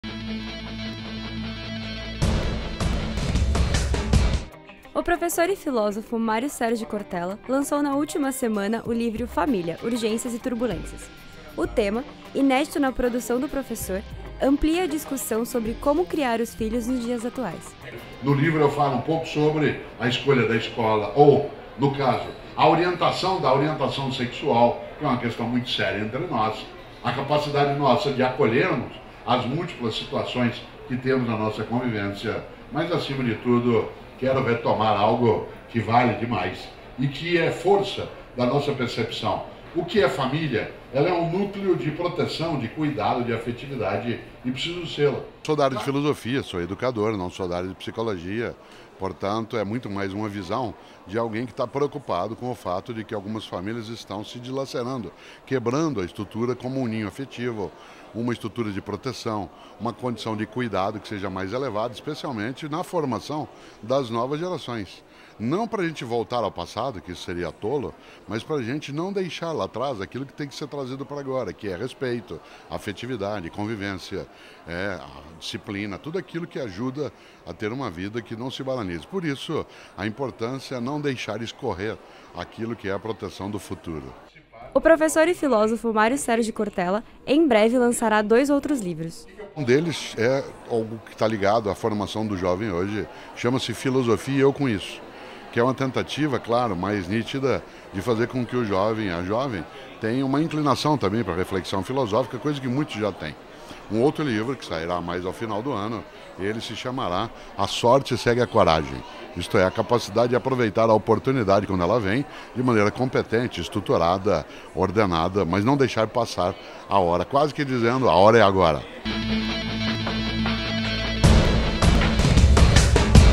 lançamento do livro do professor e filósofo Mario Sergio Cortella
O professor e filósofo Mario Sergio Cortella lançou o livro Família: Urgências e turbulências pela Cortez Editora.
O tema é inédito na produção do professor Mario Sergio Cortella e amplia a discussão sobre como criar os filhos nos dias atuais.